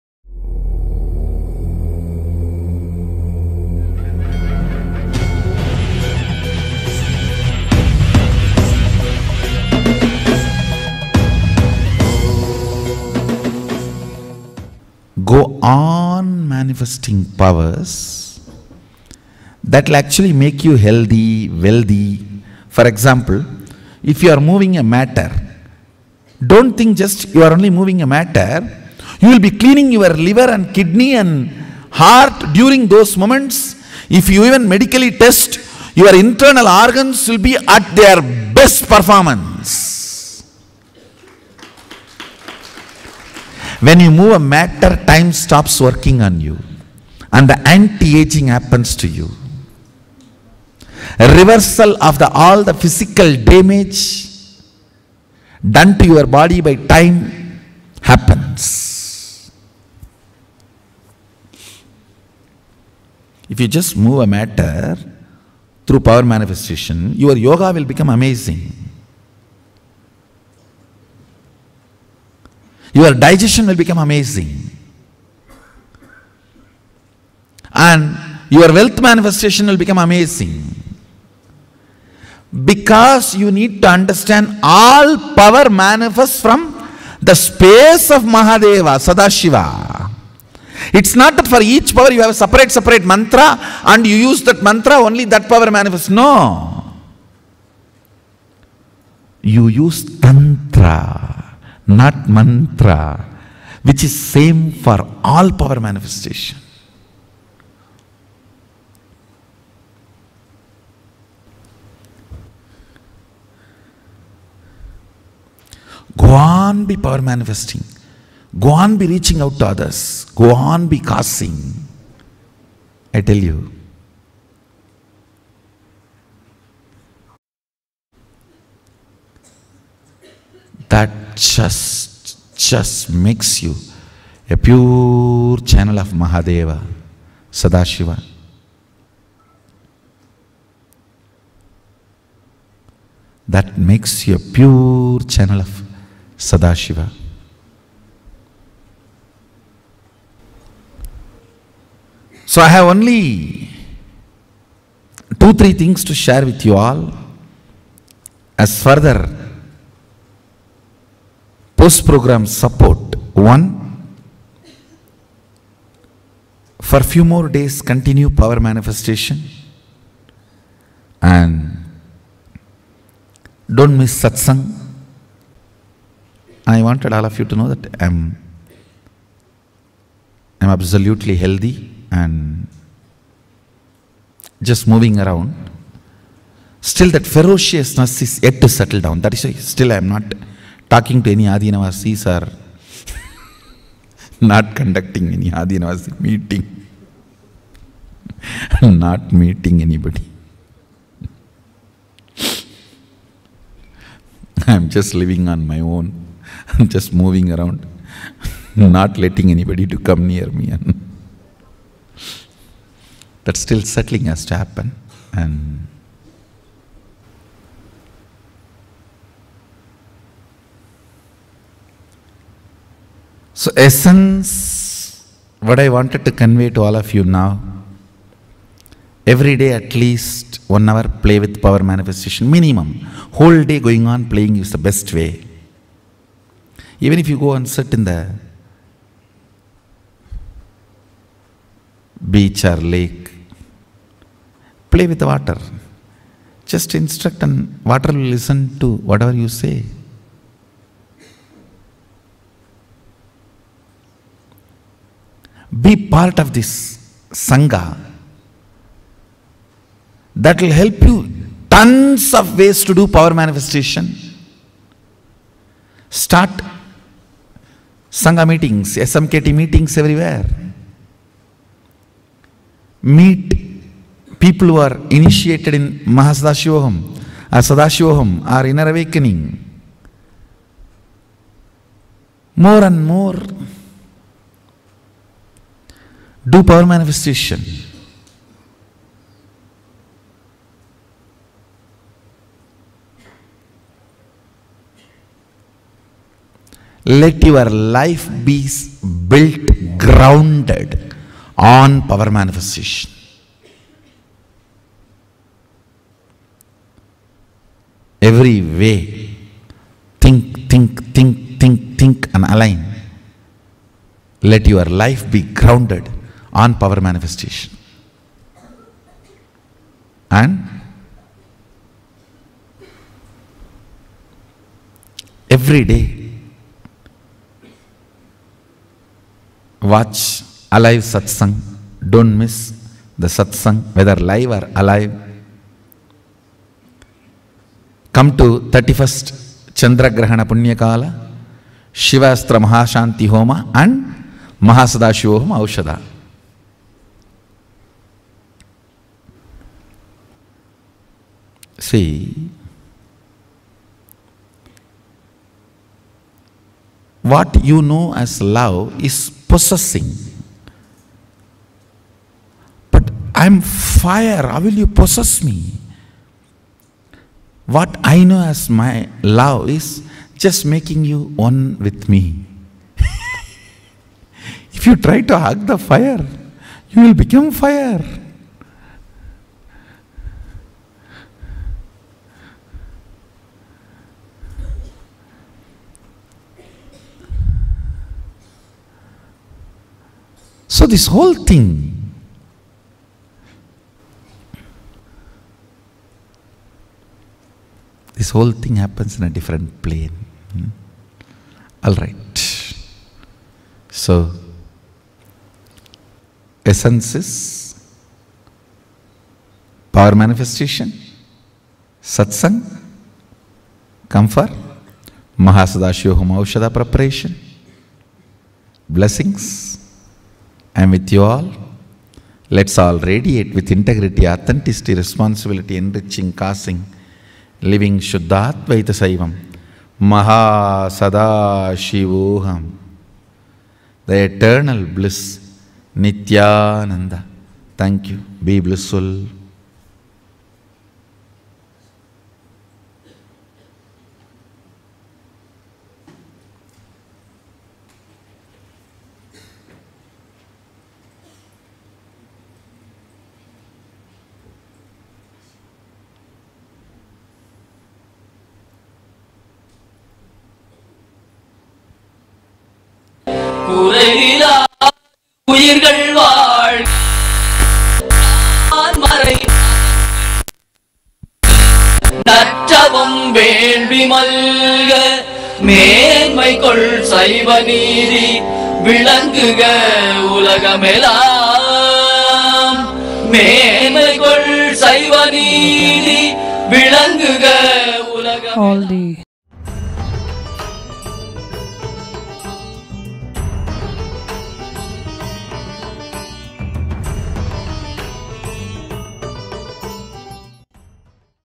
Manifest Powers & Live Royally! || Part 3 || MSS C.E || 08 Jan 2018
Name Of The Convention : Mahasadashivoham C.E
Session on: Manifest Powers & Live Royally!
Date : 08 January 2018
Venue : Adi Kailasa, Bengaluru, Karnataka, India

In this video, we see the Supreme Pontiff of Hinduism HDH Nithyananda Paramashivam expand on the science of manifesting powers. We learn that when you are able to move matter, the effect of time is removed from you. Your digestion improves, your Yoga improves, your wealth manifestation improves. We are reminded by His Divine Holiness that all powers manifest from the space of Mahadeva! If we continue to reach out to others to join and learn the science of Power Manifestation, we ourselves can become a pure channel of Mahadeva.